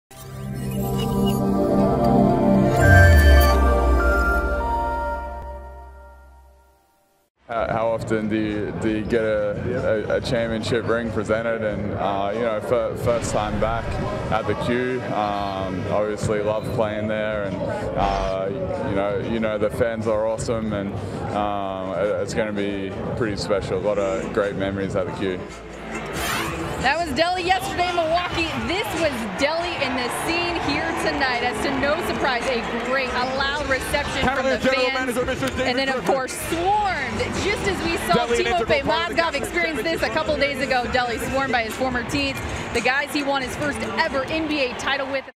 Cavs Mob Delly on His Ring Night ► 12.21.16
Check out the Cavs welcome Delly back with open arms as he is presented with his championship ring!